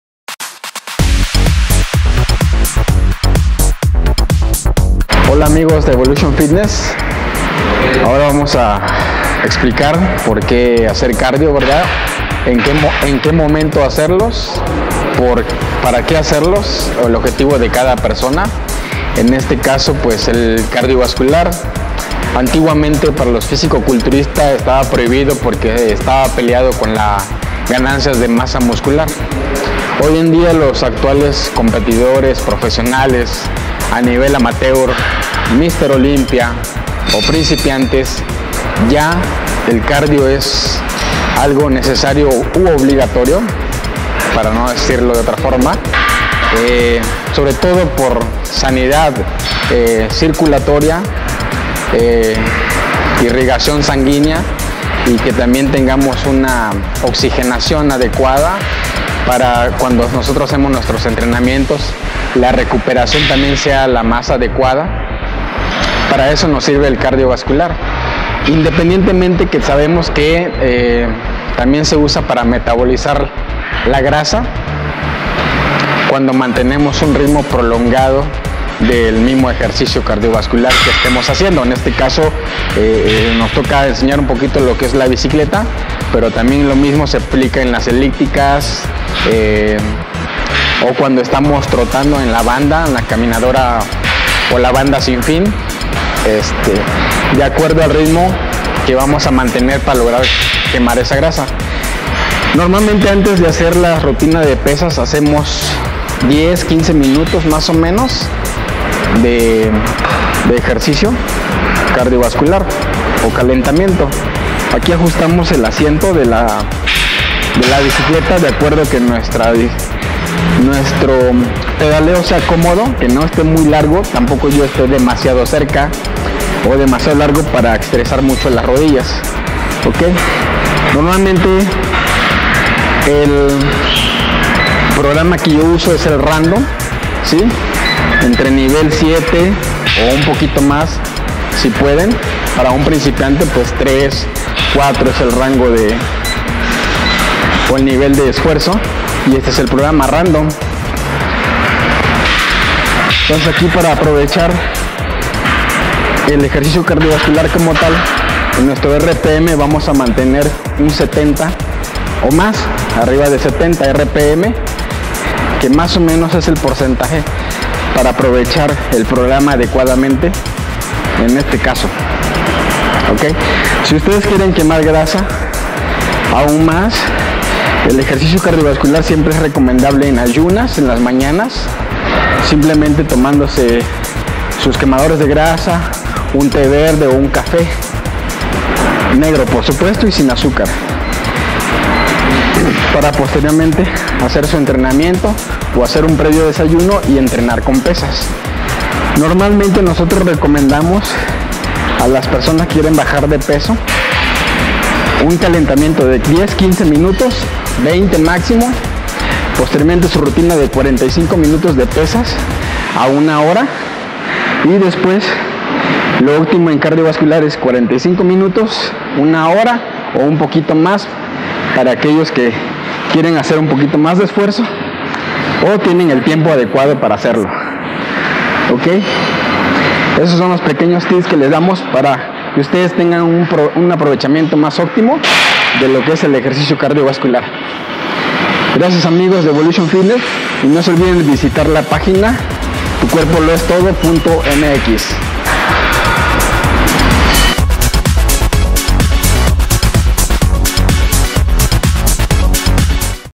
¿Cómo y cuándo hacer Cardio?
En este video, El Prof. Asunción Alcocer te explica cómo y cuándo debes hacer ejercicio cardiovascular dependiendo de tu objetivo. ¡No te lo pierdas!